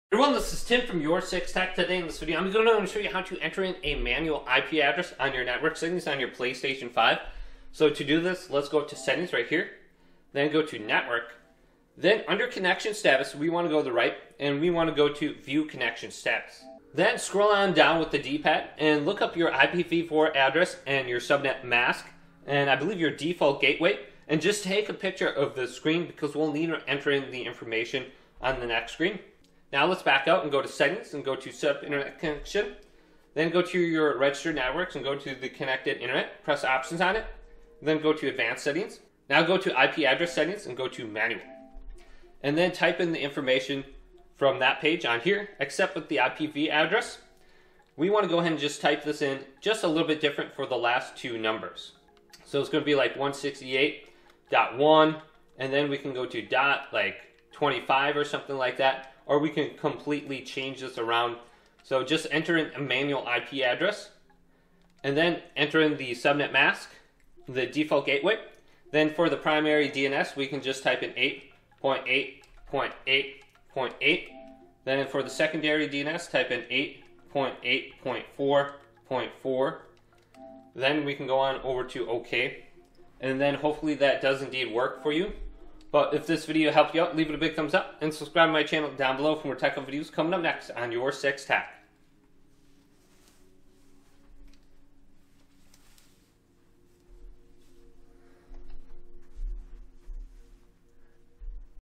How to Enter Manual IP Address on PS5 Console (Static IP)
To enter a manual IP address on your PS5 console, you can follow these steps:

Turn on your PS5 and sign in to your account to get to the main menu.
Open the system settings by selecting the gear icon on the top right of the screen.

Select Network from the list.

Highlight Settings and select Set Up Internet Connection.
Once the system completes its scan, press the options button.
In the popup menu, navigate to IP Address Settings and select Manual.
Enter your desired IP address, subnet mask, and default gateway.
Please note that manually entering an IP address is not recommended unless you have a specific reason for doing so1. If you are unsure whether you should enter a manual IP address, please contact your internet service provider for assistance.